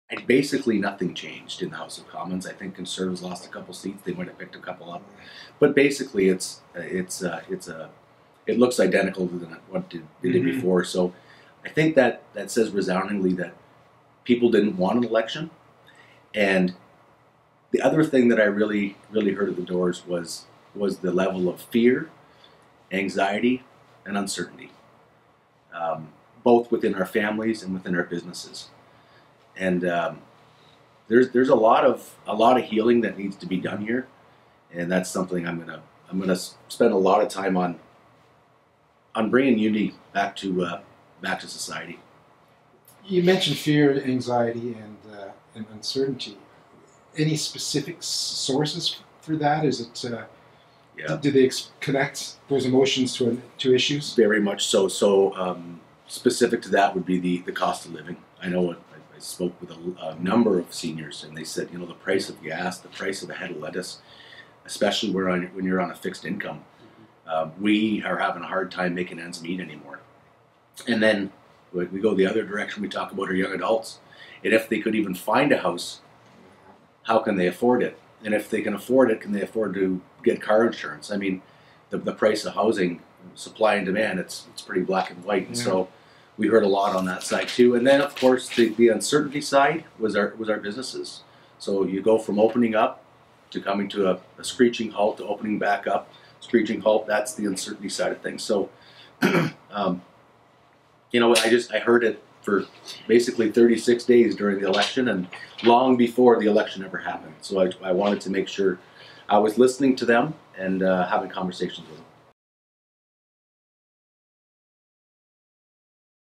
Re-elected MP Chris Lewis eager to tackle second term
MP Chris Lewis who was re-elected in the riding of Essex speaks with Windsor Star reporter Dave Waddell on Thursday, September 23, 2021.